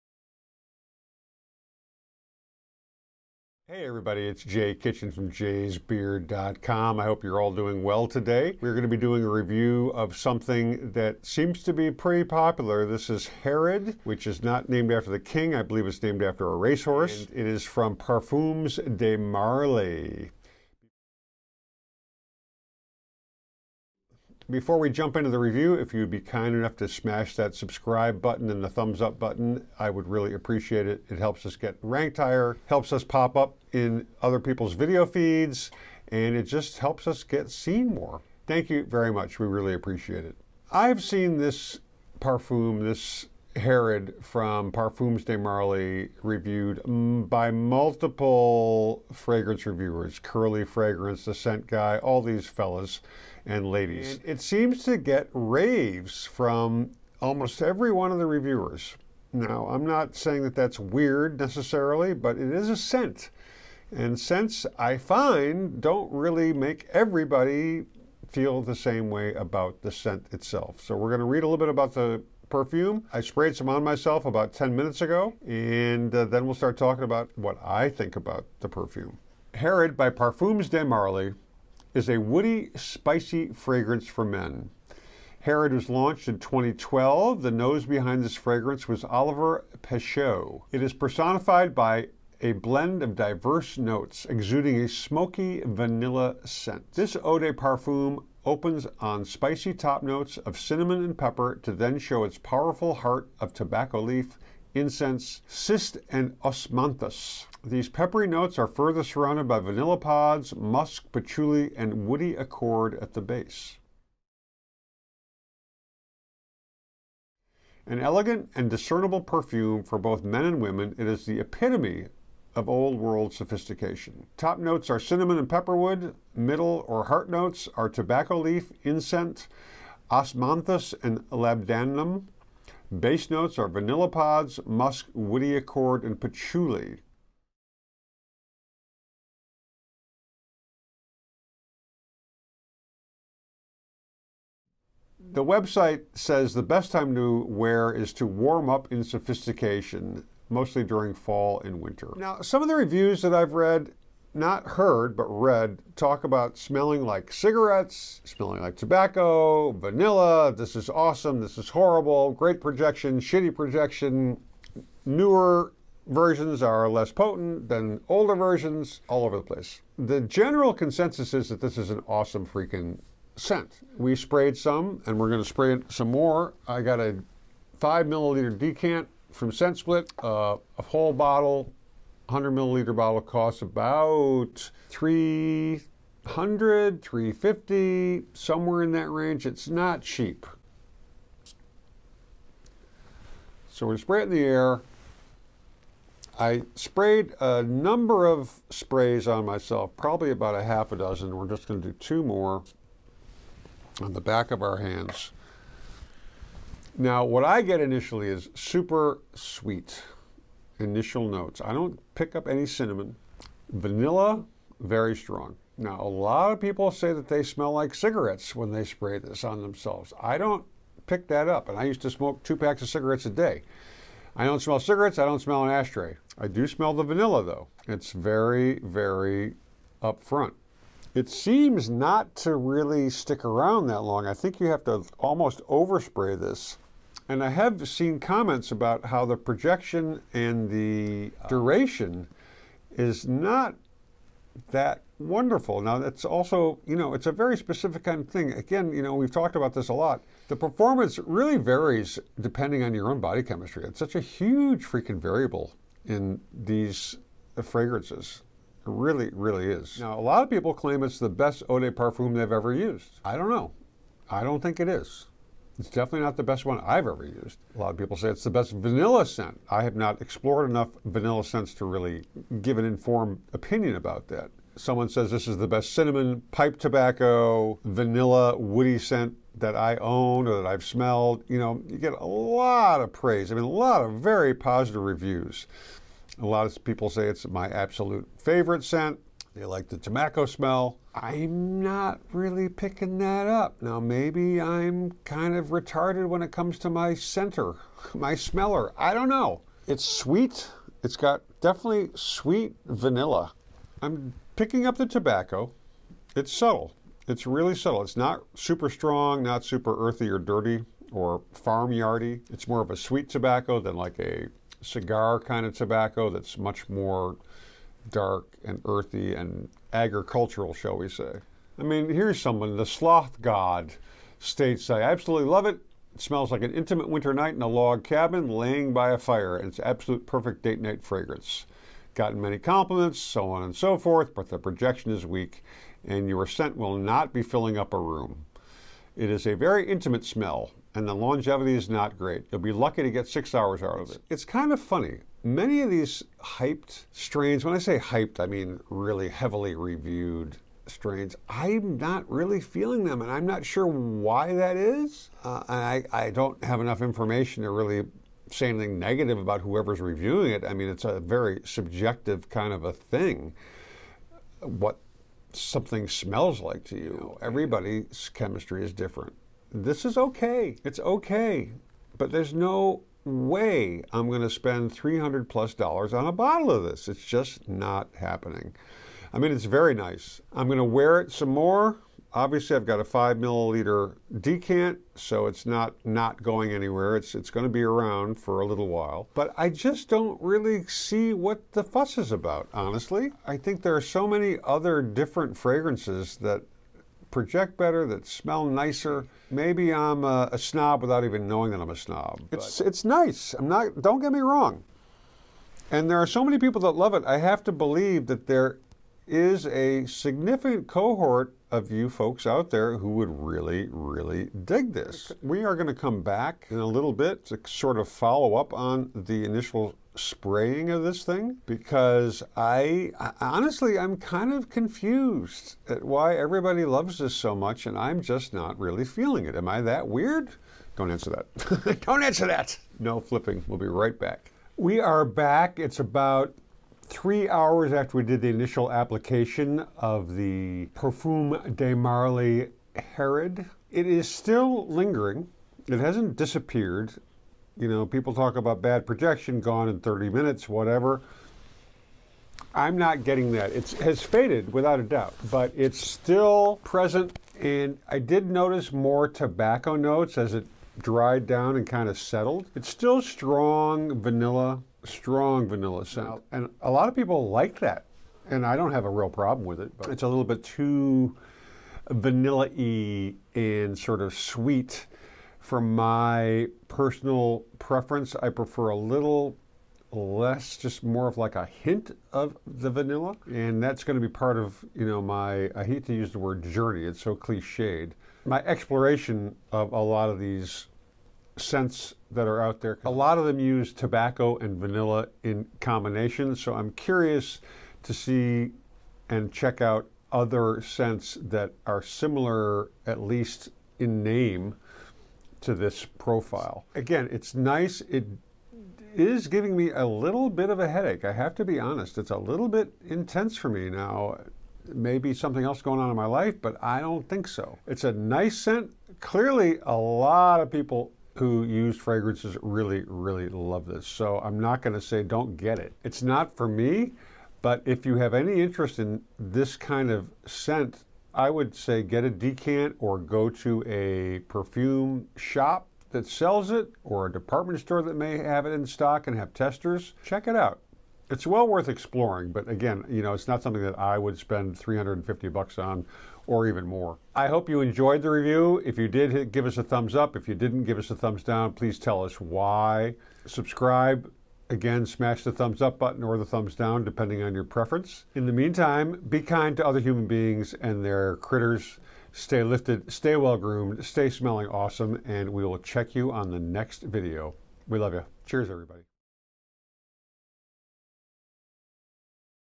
Parfums de Marly Herod Why is this so Loved?
Parfums de Marly Herod

Herod by Parfums de Marly is a Woody Spicy fragrance for men. Herod was launched in 2012. The nose behind this fragrance is Olivier Pescheux.

Herod is personified by its blend of diverse notes, exuding a smoky vanilla scent.

This eau de parfum opens on spicy top notes of cinnamon and pepper to then show its powerful heart of tobacco leaf, incense, ciste and osmanthus.

These peppery notes are further surrounded by vanilla pods, musk, patchouli and woody accord at the base.

An elegant and discernible perfume, for both men and women, it is the epitome of old-world sophistication.

Top notes are Cinnamon and Pepperwood™;

middle/heart  notes are Tobacco Leaf, Incense, Osmanthus and Labdanum;

 base notes are Vanilla pods, Musk, Woody accord, patchouli

Best time to wear: To warm up in sophistication, during fall and winter

INGREDIENTS

Alcohol Denat., Parfum (Fragrance), Aqua (Water), Coumarin, BHT, Benzyl Benzoate, Linalool, Geraniol, Limonene, Cinnamal, Eugenol